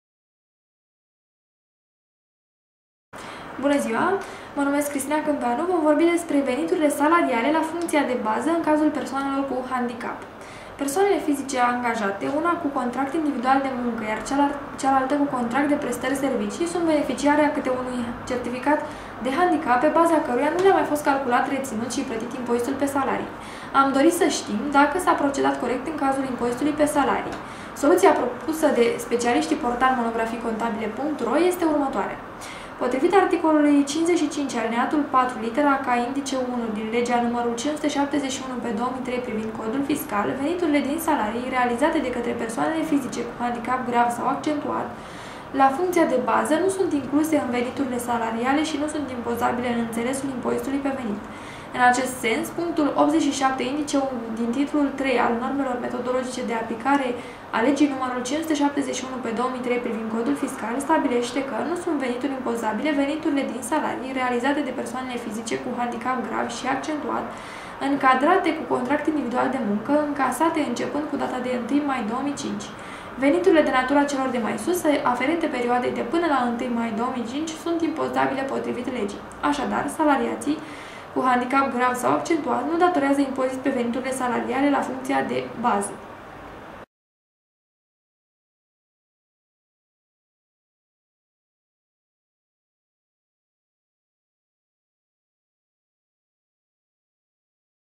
Persoane cu handicap. Veniturile salariale la functia de baza
Persoane fizice angajate, una cu contract individual de munca, iar cealalta cu contract de prestari servicii, sunt beneficiare a cate unui certificat de handicap pe baza caruia nu le-a mai fost calculat, retinut si platit impozitului pe salarii.

S-a procedat corect in cazul impozitului pe salarii?